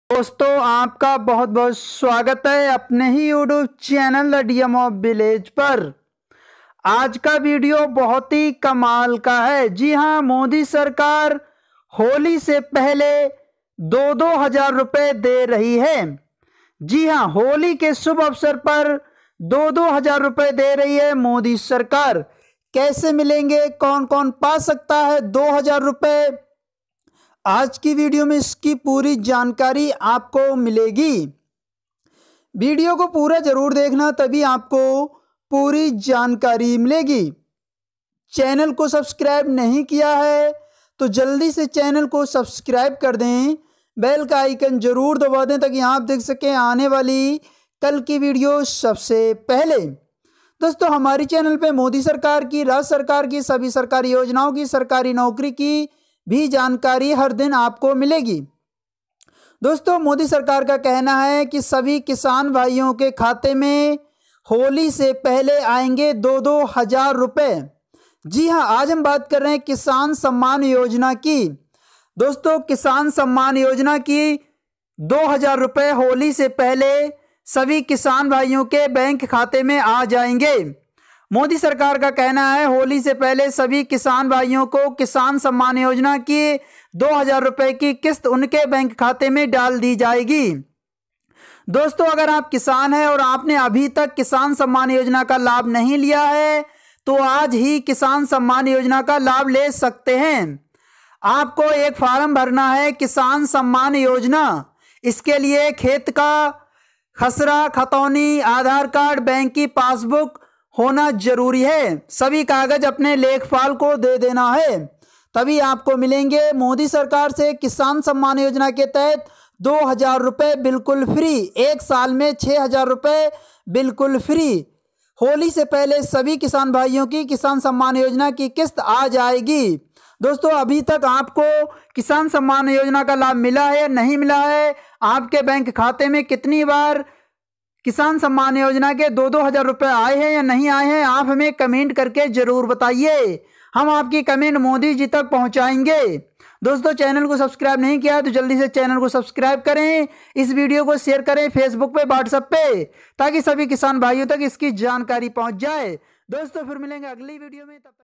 होली से पहले मिलेगा होली का तोहफा यह फार्म भर दीजिए
दोस्तों आज की वीडियो में हमने जानकारी दी है प्रधानमंत्री की किसान सम्मान योजना की प्रधानमंत्री नरेंद्र मोदी का कहना है कि प्रधानमंत्री किसान सम्मान योजना की किस्त होली पर सभी किसान भाइयों के बैंक खाते में पहुंच जाएगी किसान हेतु किसान सम्मान योजना का जरूर लाभ लीजिए वीडियो को लाइक करें वीडियो को शेयर करें चैनल को सब्सक्राइब करें धन्यवाद